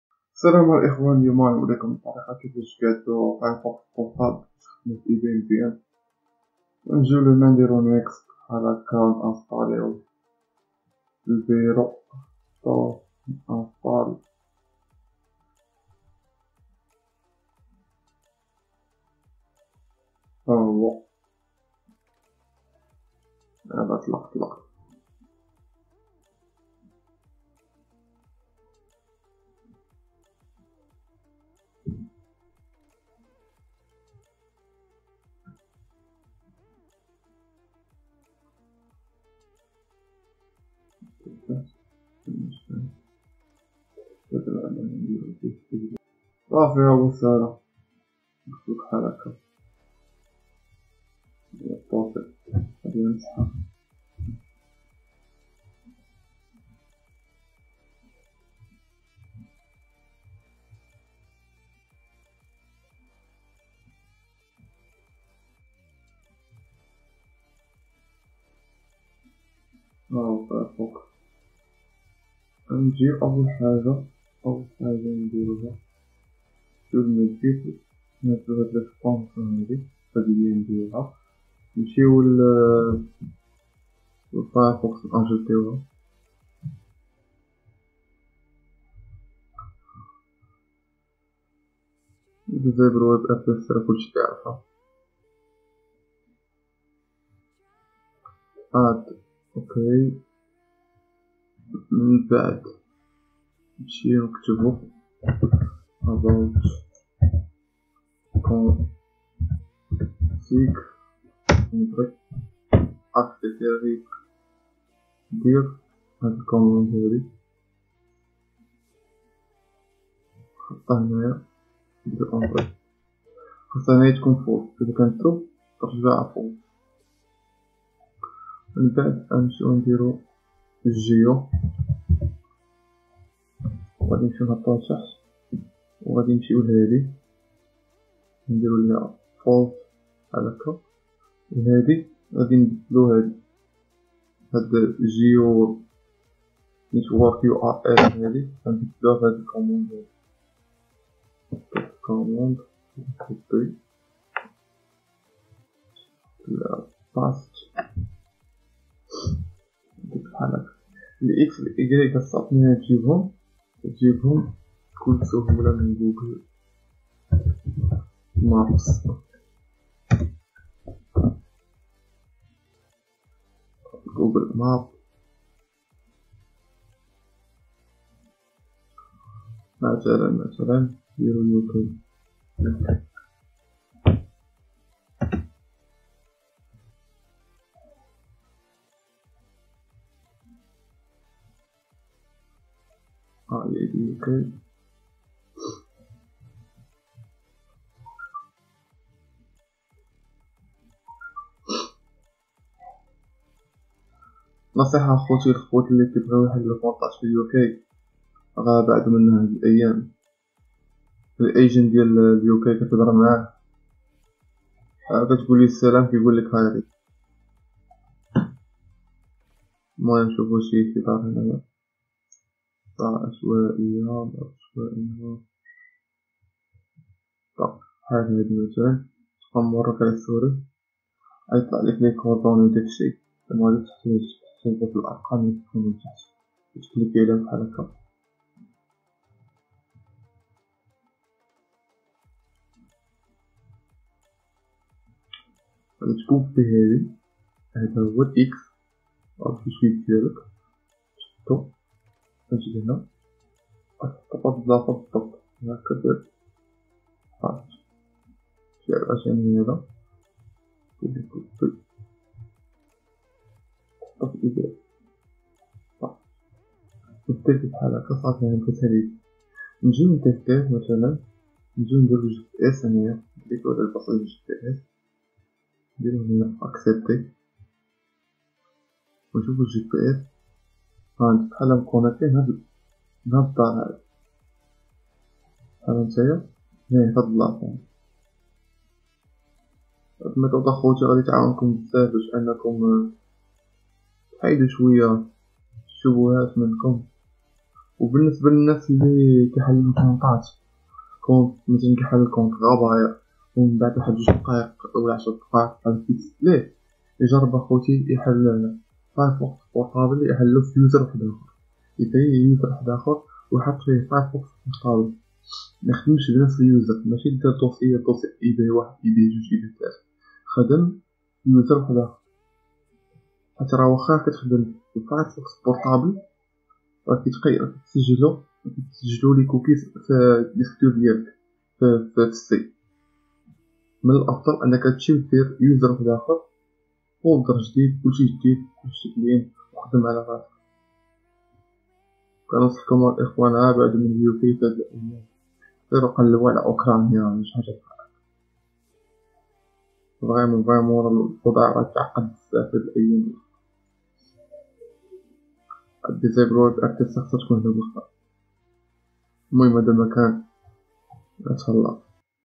شرح اعدادات متصفح فايرفوكس بورطابل كونفيك لفتح حسابات ايباي متعددة
سمحو ليا مايك خاري ولاكين هاد شي لي عطا الله

about:config \ geo

data:application/json,{"location": {"lat": x, "lng": y}}

about:config \

media.peerconnection.enabled

1:00
2:00
3:00
4:00
5:00